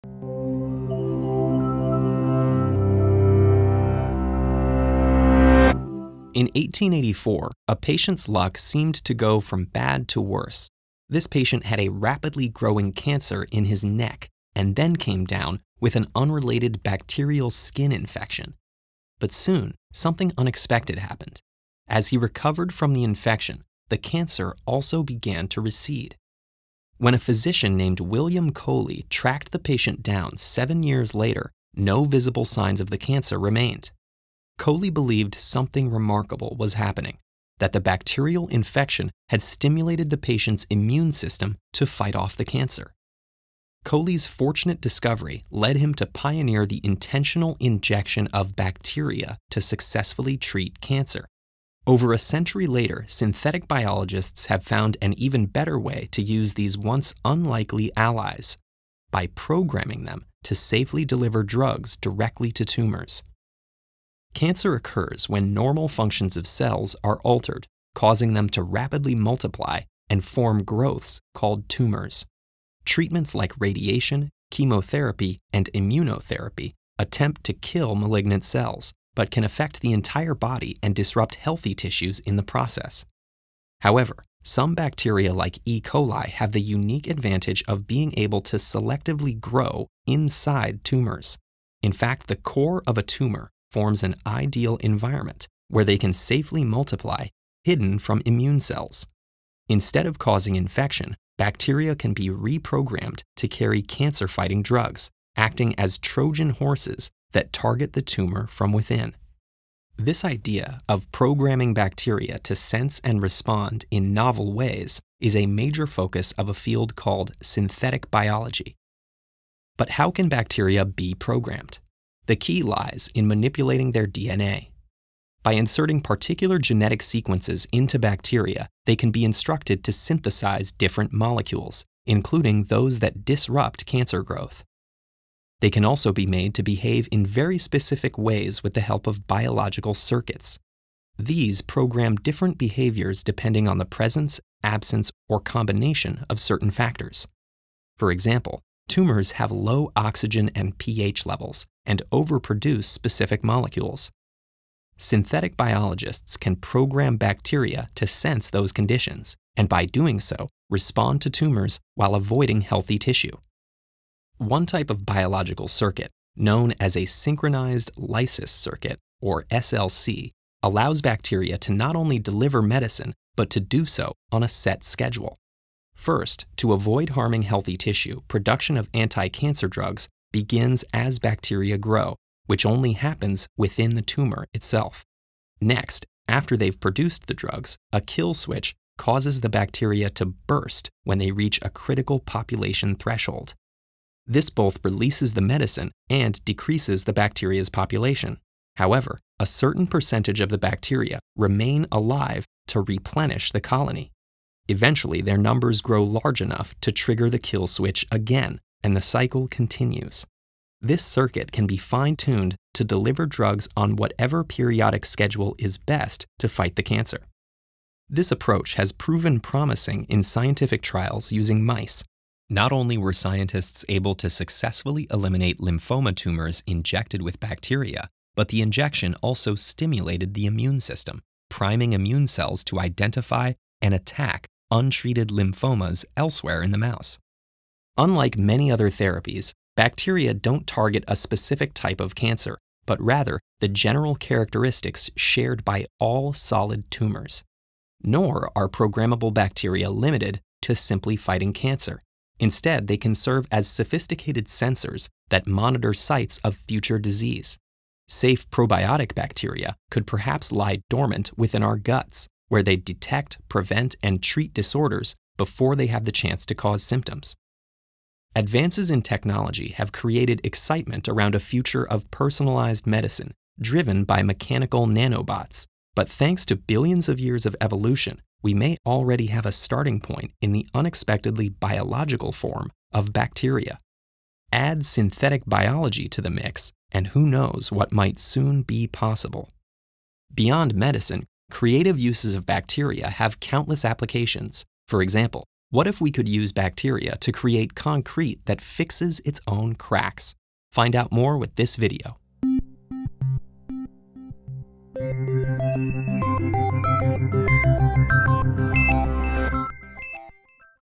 Hacking bacteria to fight cancer - Tal Danino
Explore how synthetic biologists are programming bacteria to fight cancer by manipulating their DNA.

In 1884, an unlucky patient who had a rapidly growing cancer in his neck came down with an unrelated bacterial skin infection. As he recovered from the infection, the cancer surprisingly began to recede. The infection had stimulated the patient’s immune system. Today, synthetic biologists program bacteria to safely deliver drugs directly to tumors. How is this possible? Tal Danino investigates.

Lesson by Tal Danino, directed by Chris Bishop.

Thank you so much to our patrons for your support! Without you this video would not be possible!